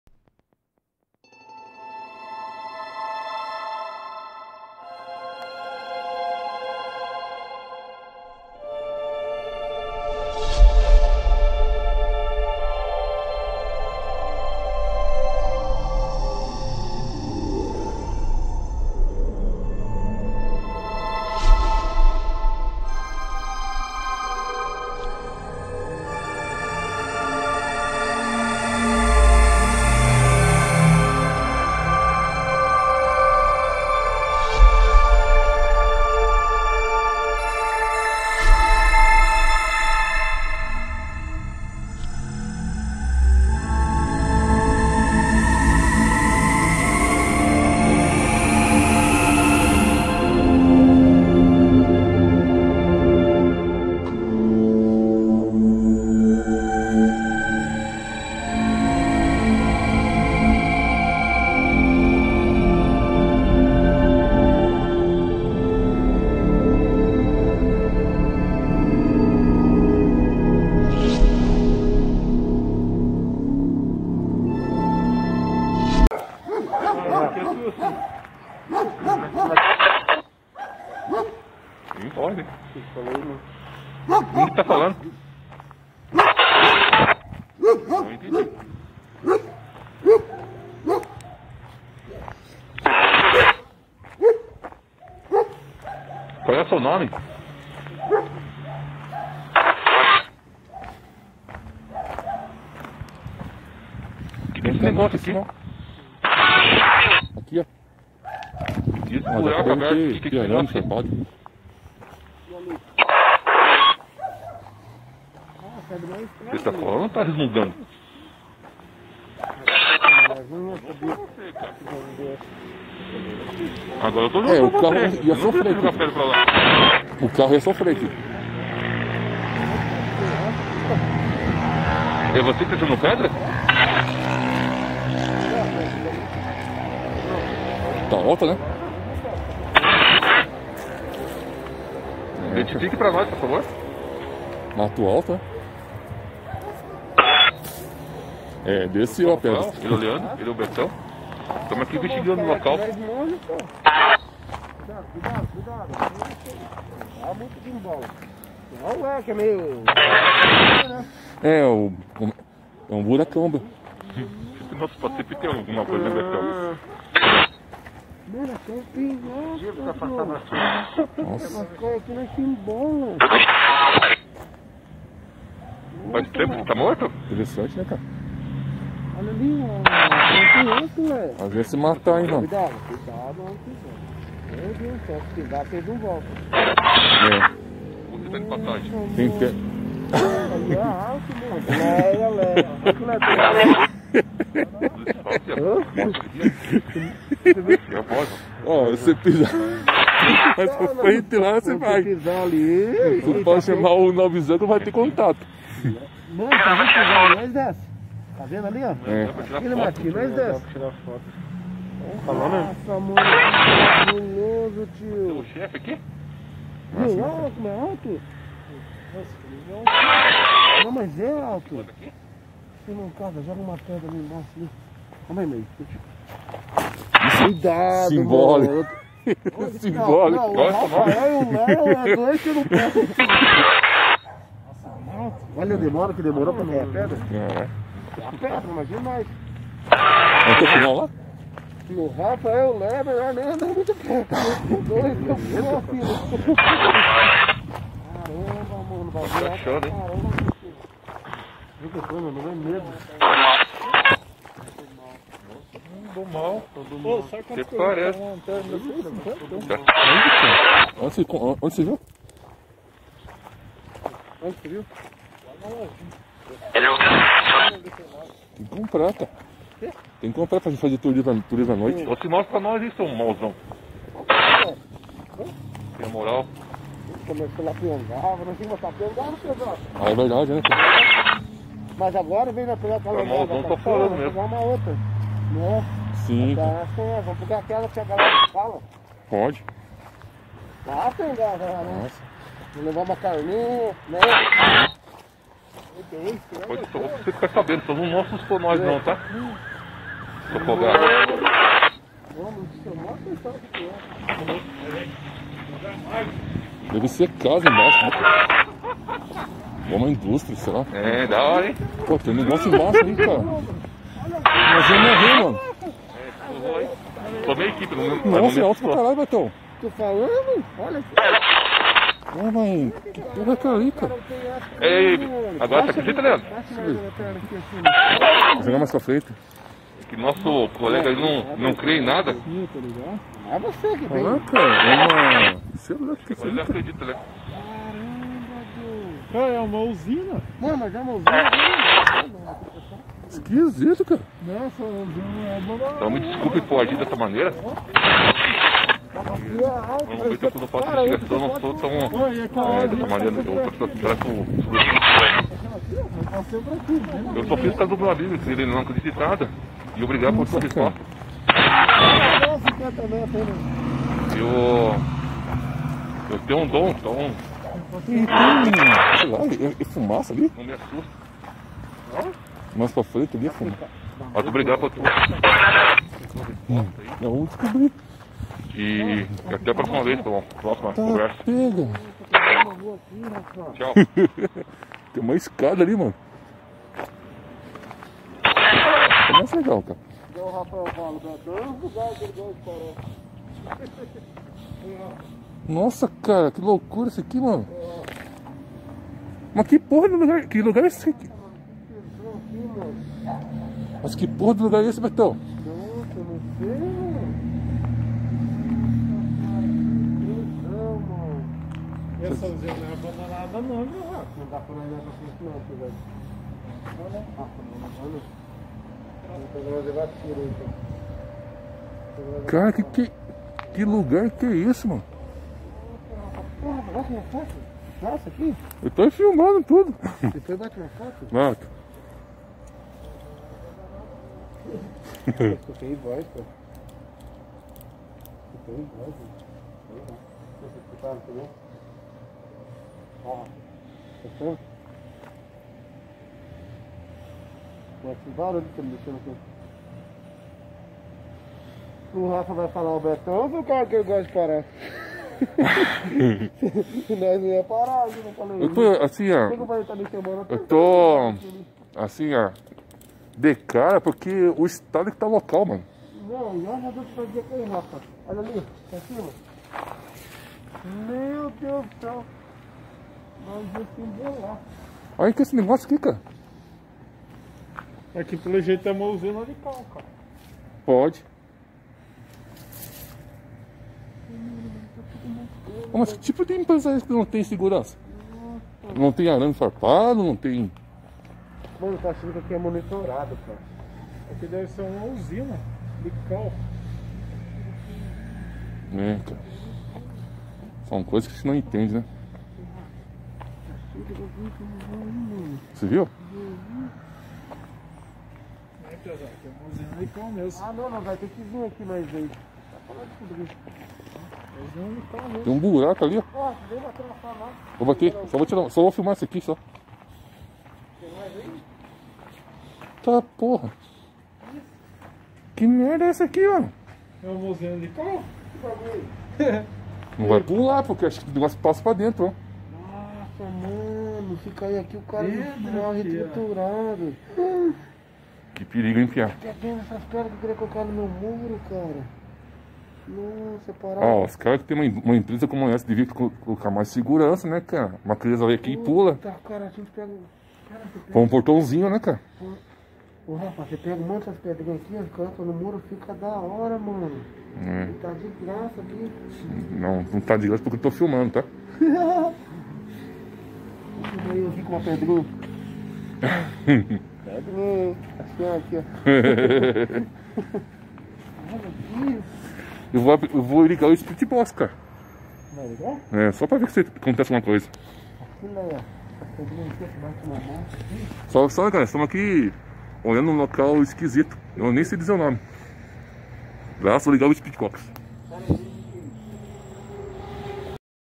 Moinho da Morte em Colombo PR
Moinho da Morte

Empresa Abandonada no meio do nada, sem segurança e com risco a vida de todos que passam pelo local. No deslocamento do Mistério ao horizonte sem destino, eu YouTuber Rafael Crispim Bezerra,  Inscrito Betão  e Inscrito Leandro, encontramos uma empresa abandonada se serviços desconhecidos com atividades sobrenaturais e paranormais.

Vem conosco no rumo do Mistério...